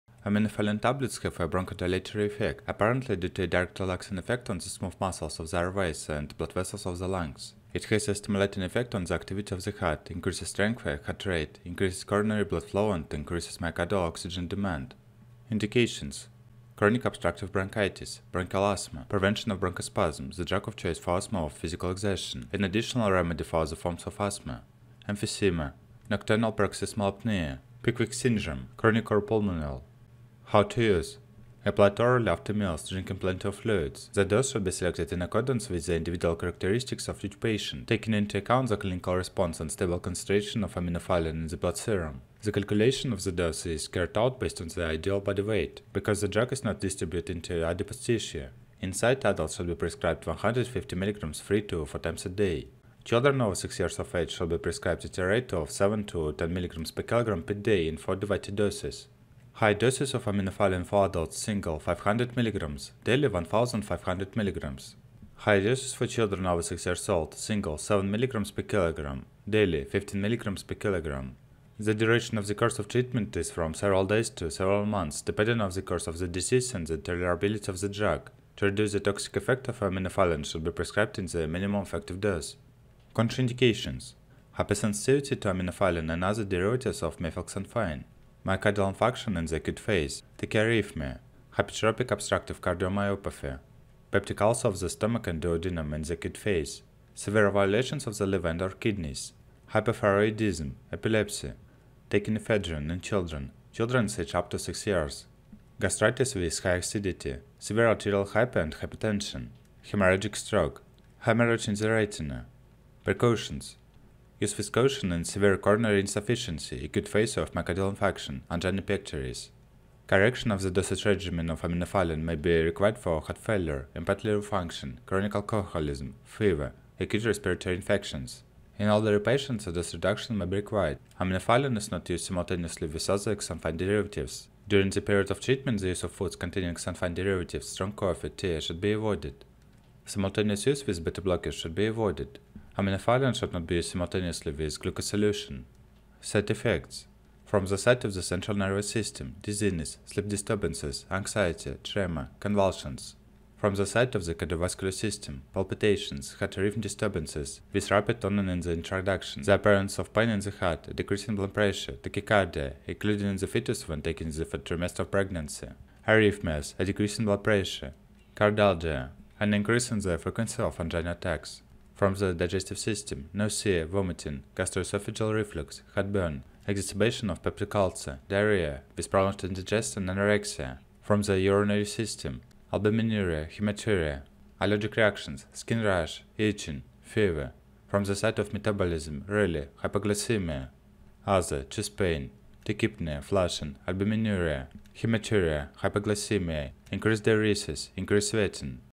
Aminophylline tablets (Euphylline) how to use: Uses, Dosage, Side Effects, Contraindications
Aminophylline tablets have a bronchodilatory effect, apparently due to a direct relaxing effect on the smooth muscles of the airways and blood vessels of the lungs.

Video Content:
0:00 Aminophylline tablets (Euphylline)
0:15 Indications
0:30 How to use
1:22 Contraindications
1:52 Precautions
2:25 Side effects